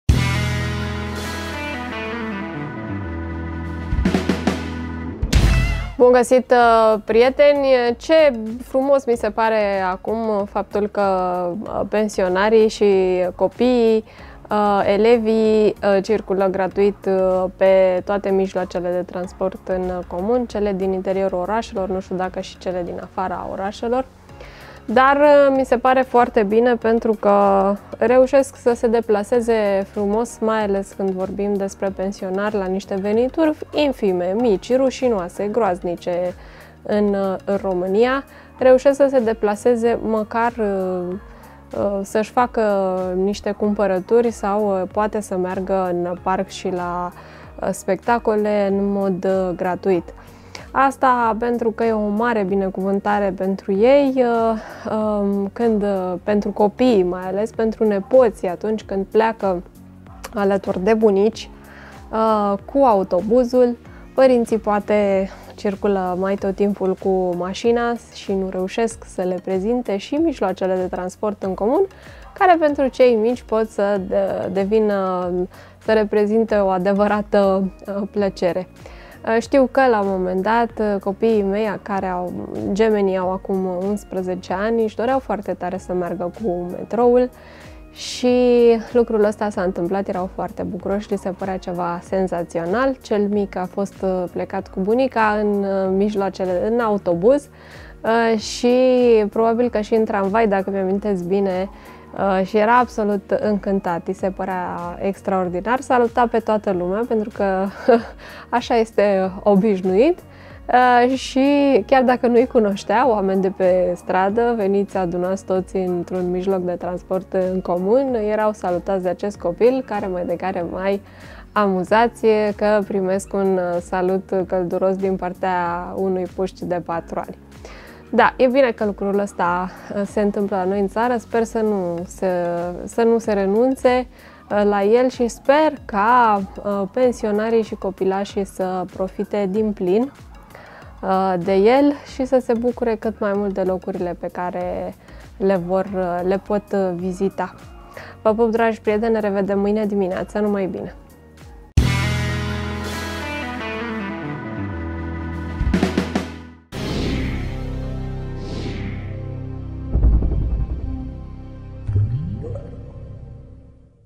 Bunicii își plimbă nepoții gratis prin București.
De vorba cu Iulia! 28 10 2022